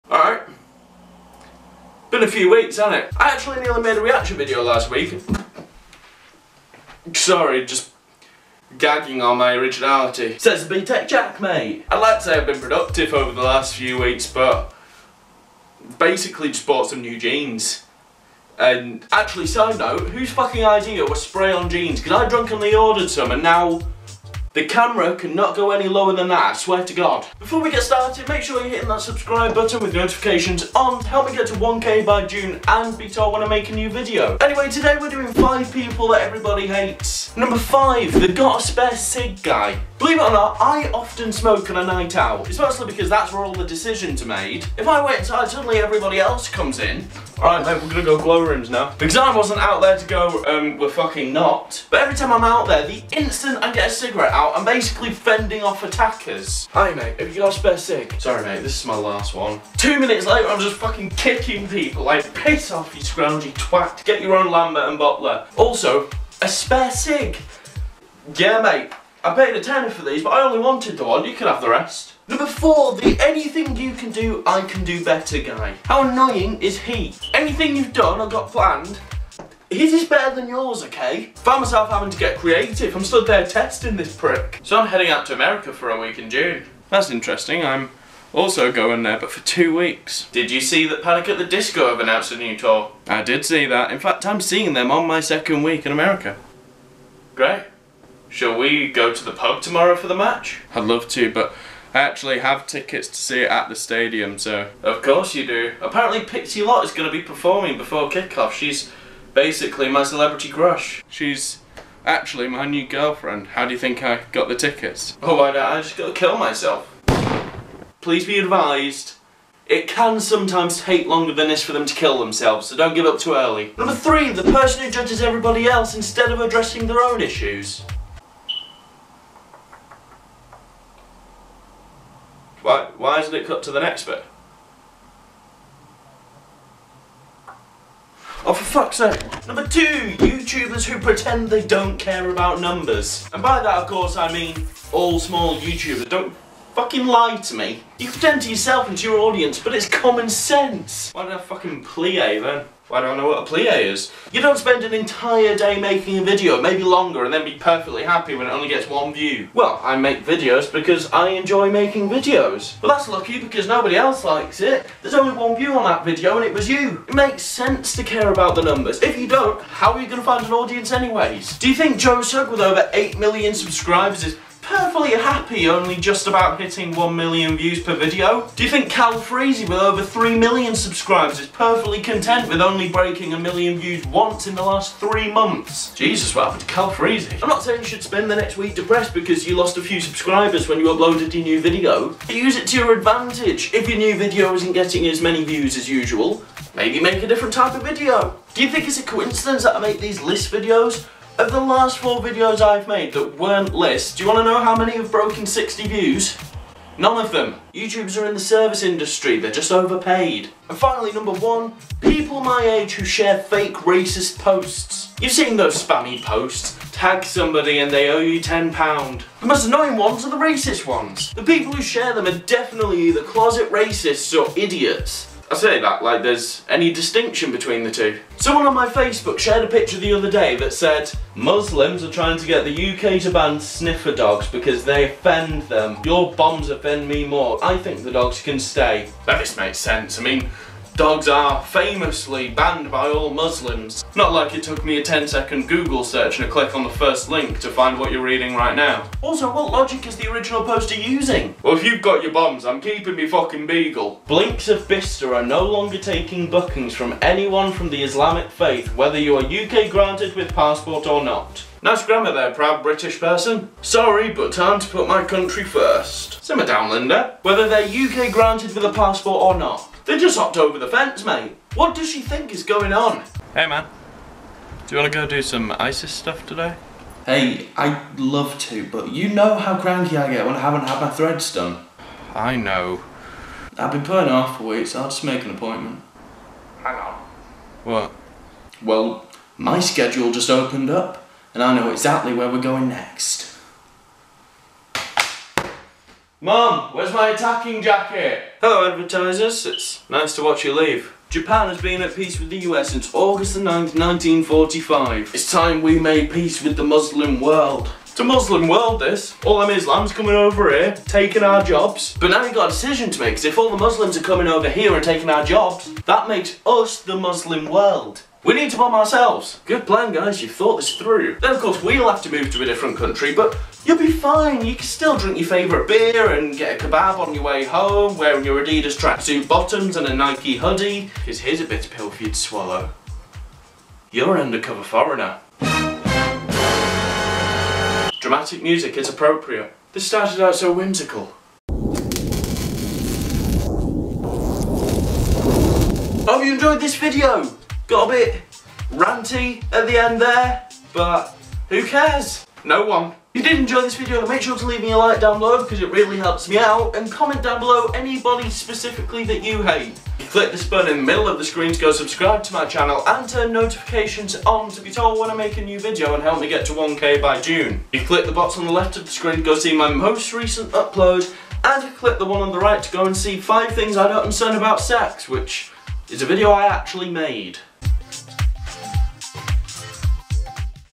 5 PEOPLE EVERYBODY HATES *racism rant*
It's another sarcastic list video, alert the media!

If you enjoyed this video then click like below!

If you're reading this then comment any type of person you hate!

steviedav